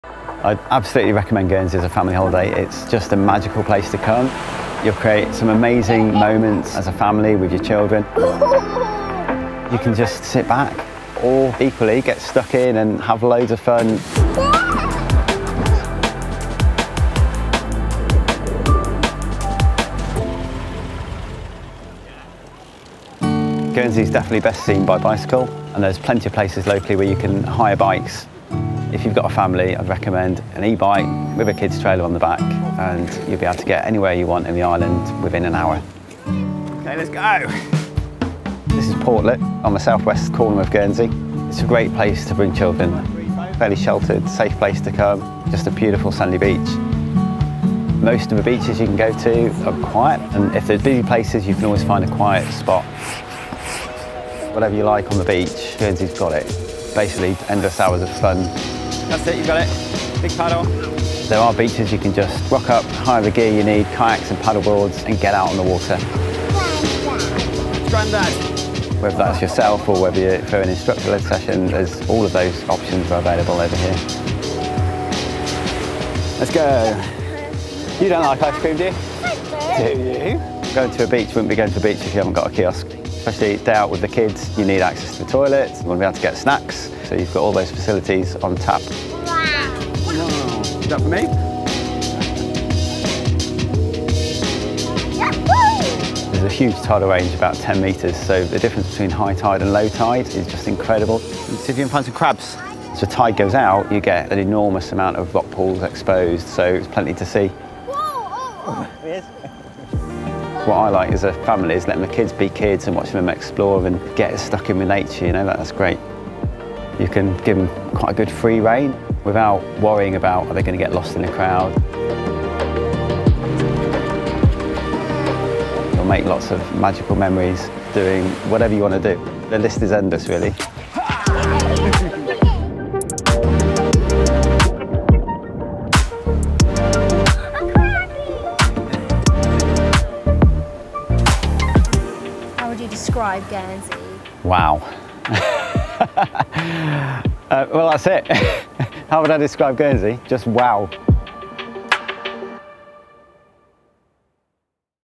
Family Beach Day with Chris
Get insider travel tips from the locals who know.

Meet the locals who want to show you their Guernsey, so that you can have a truly authentic experience on your next visit to our Islands.

Chris is a local teacher and family man who spends all of his spare time outside with his family exploring on both land and sea. Join Chris and his family on an active afternoon out to see just how much fun you can have with kids on the Islands.